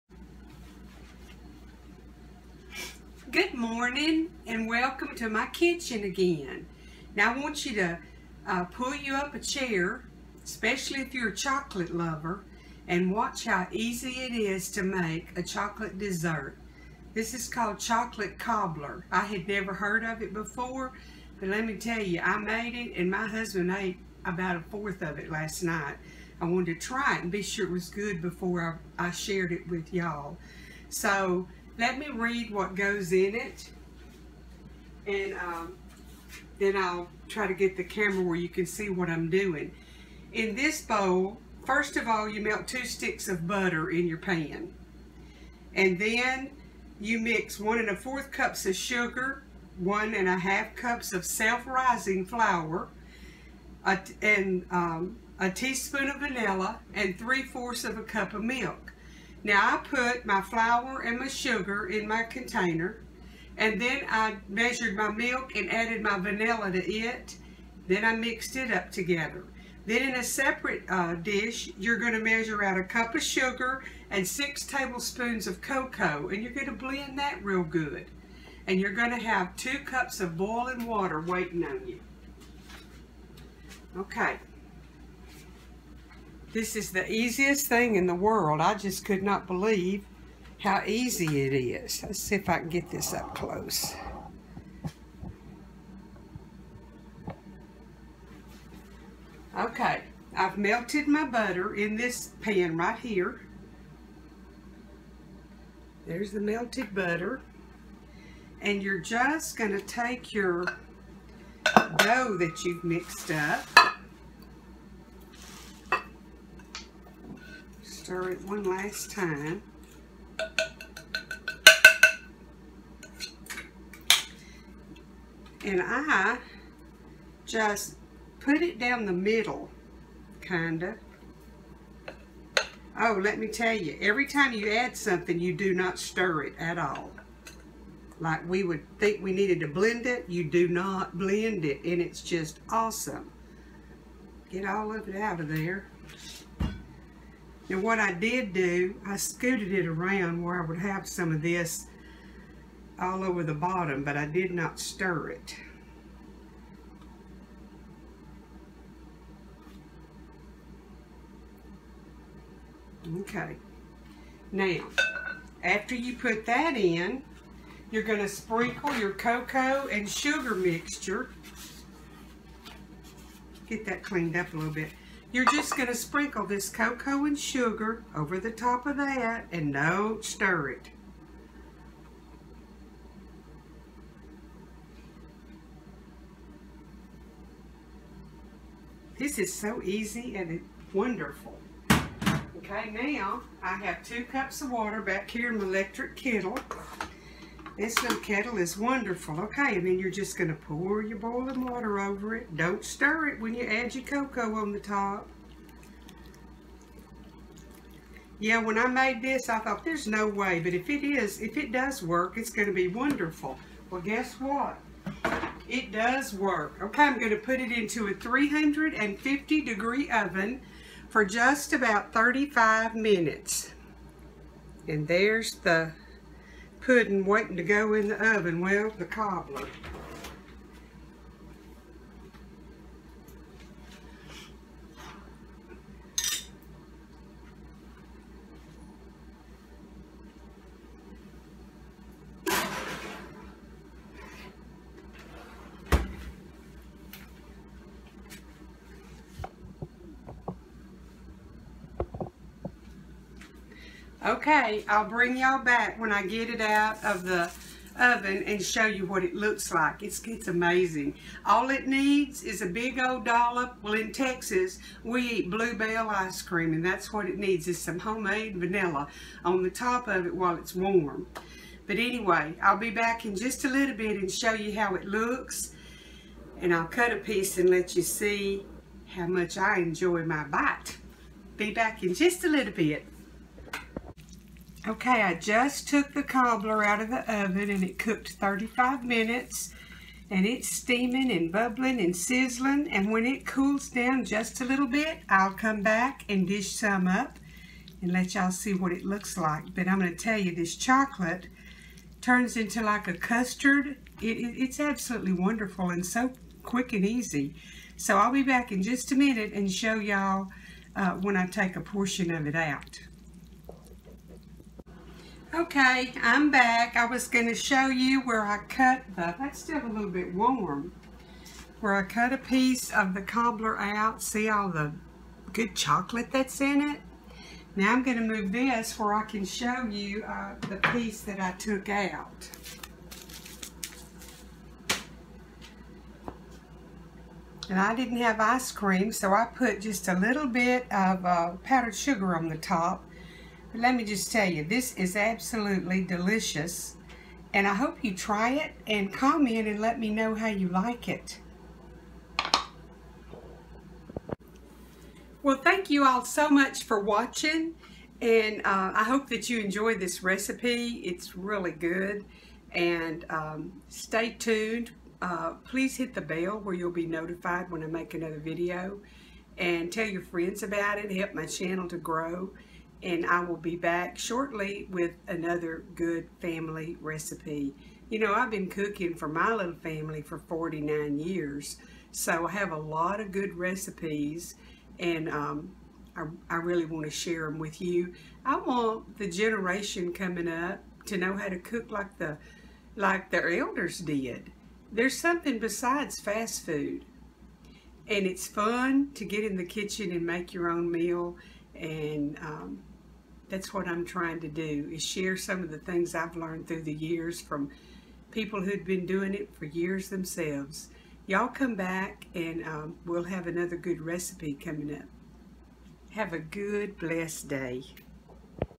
#9 Chocolate Cobbler
Easy, decadent, quick to prepare chocolate dessert that will have most folks begging for a second helping! Chocolate lovers unite!Try it...… you'll like it!!!

link to my thrive life freeze dried food page